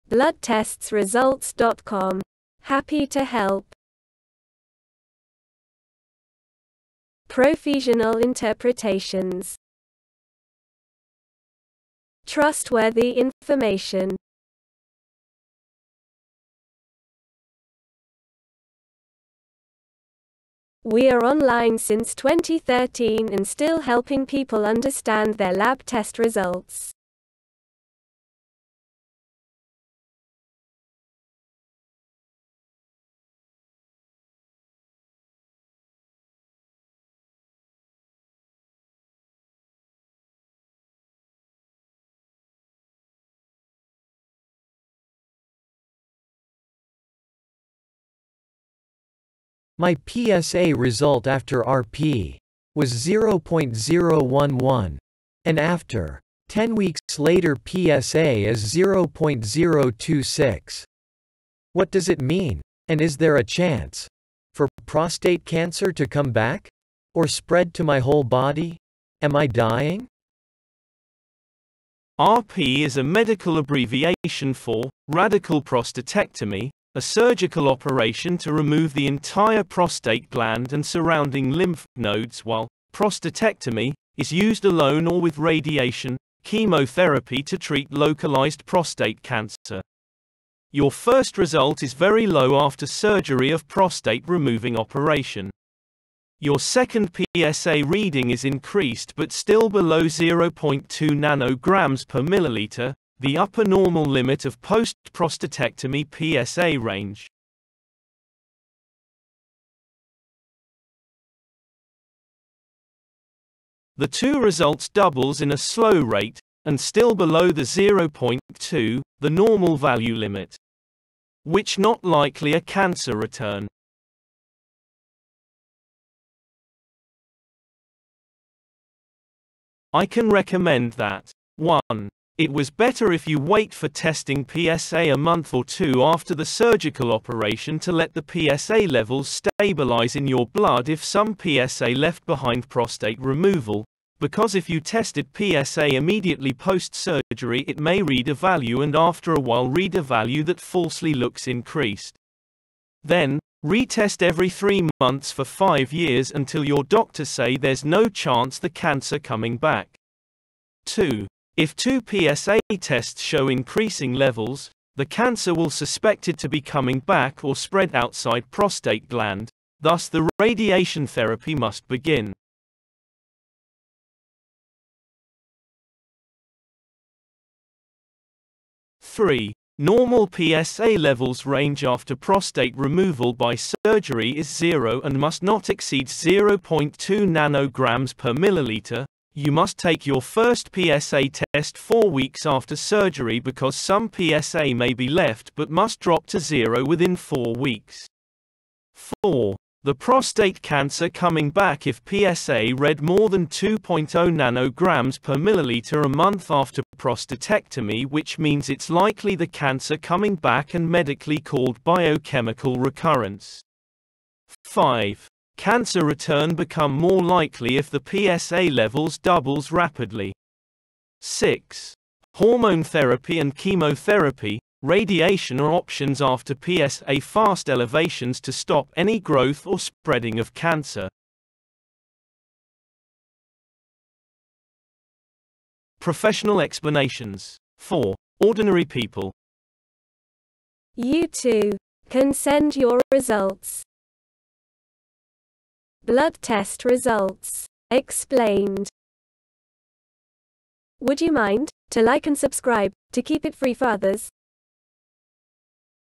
My PSA result after RP was 0 011 and after 10 weeks PSA is 0 026 What does it mean, is prostate can
My PSA result after RP
was 0.011 and after
10 weeks later PSA is 0.026
And is there a chance for prostate cancer to come back or spread to my whole body? Am I dying?